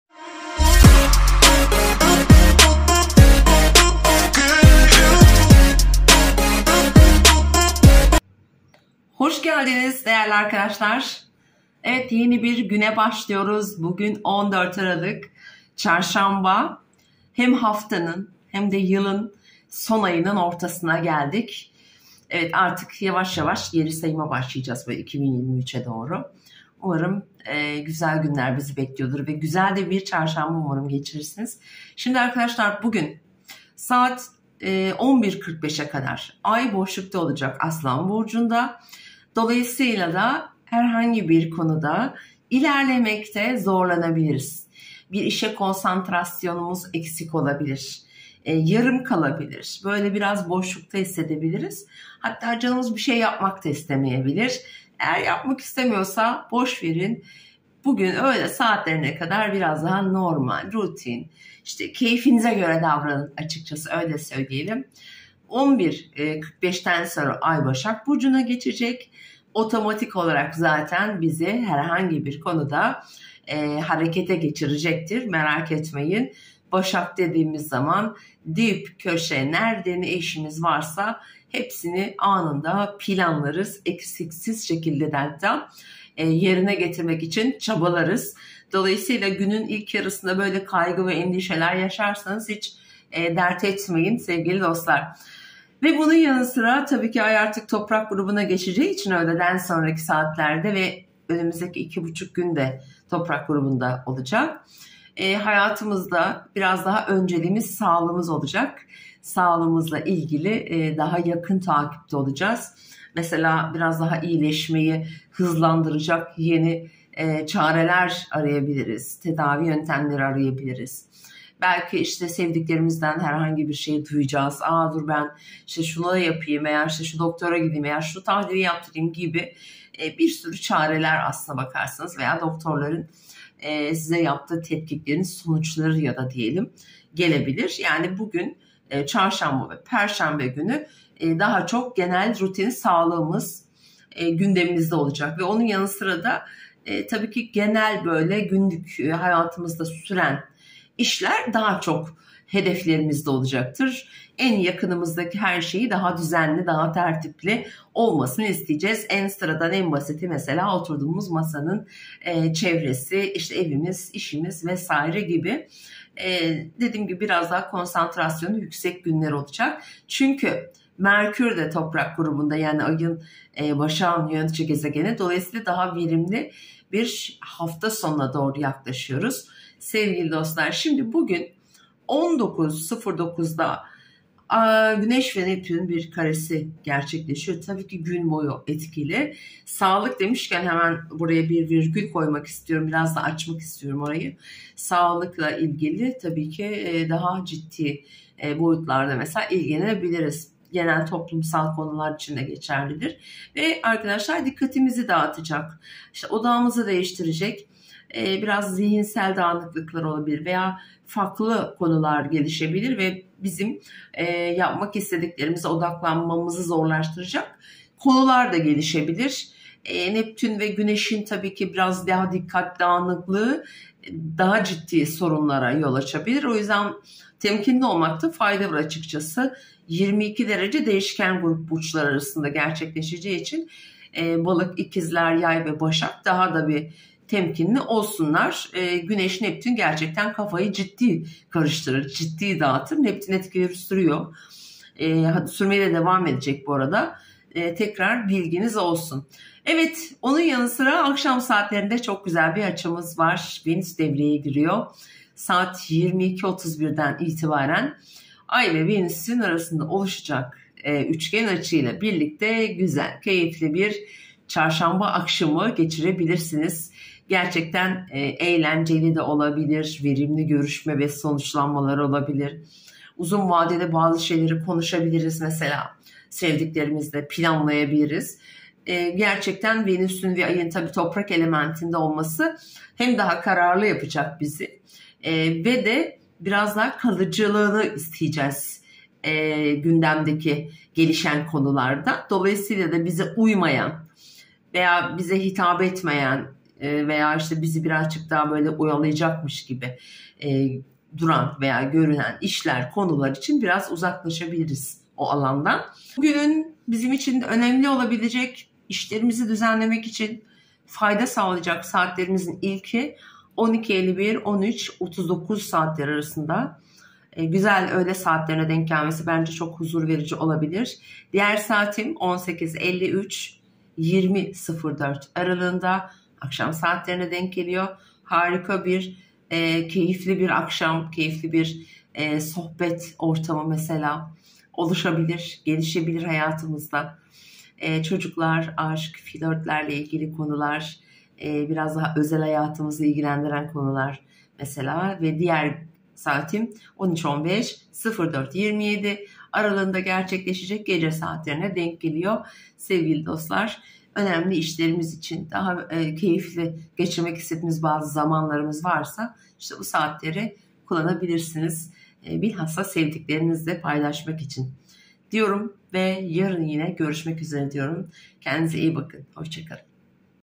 14 Aralık 2022 Günlük Astroloji Yorumu.Ay Öğle saatlerine kadar boşlukta.
14 Aralık 2022 çarşamba
Haftaya Ay Aslan burcunda başlıyoruz. Kendini ortaya koyanlar, dikkat çekecek ve çok konuşulacak.

kasımayı Yeni Ay, niyetlerimizi planladığımız ve evrene hayallerimizin tohumlarını ektiğimiz evredir.